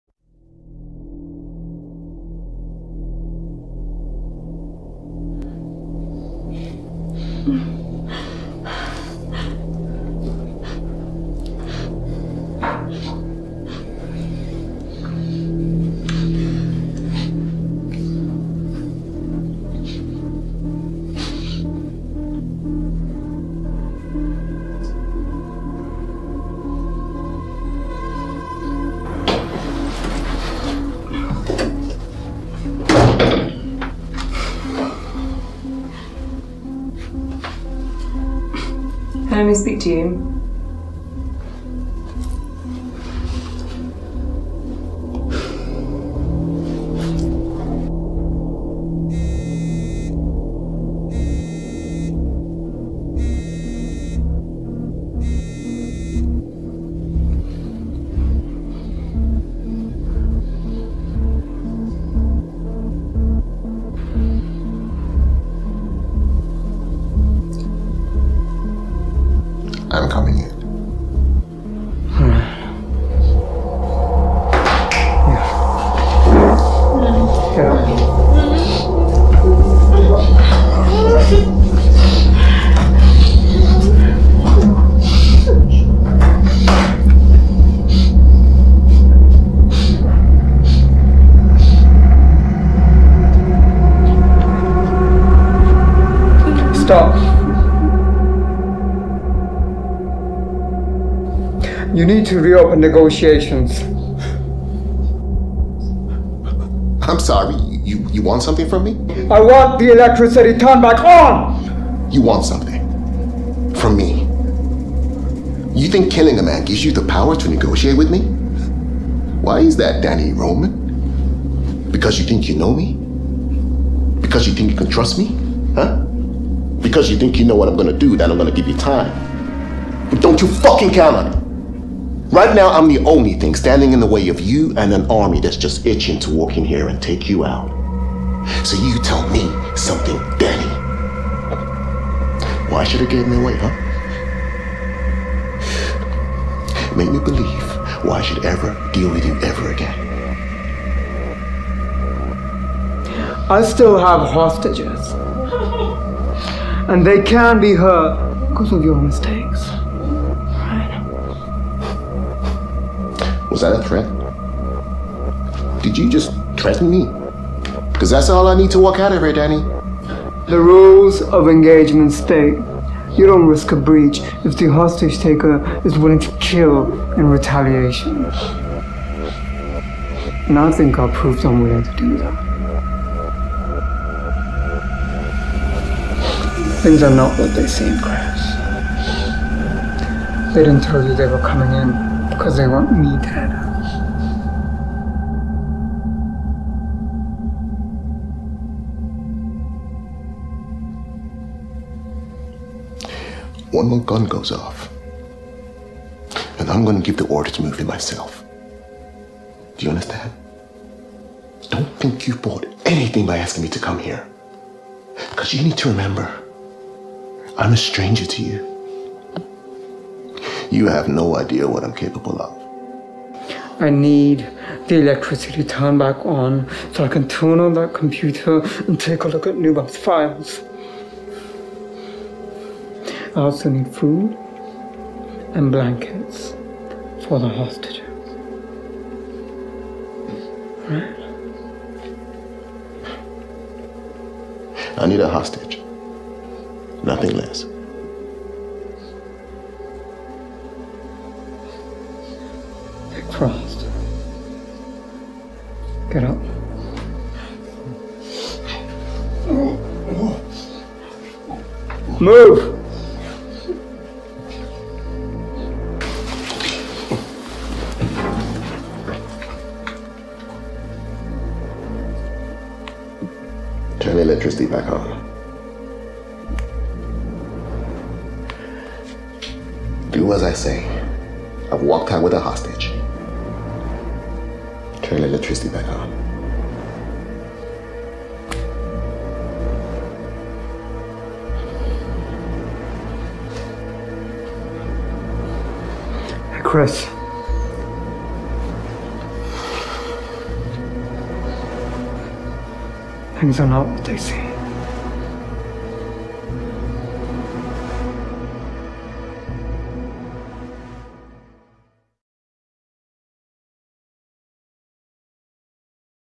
Interrogation Scene Jan 2019
Shot & Edited by Luke Ayres
Starring Sid Khanna, Patrick Regis, Natalie Timoshenko, Camila Piccinini, Katherine Rose, Steve Barlow.

An Imperial Buddha Production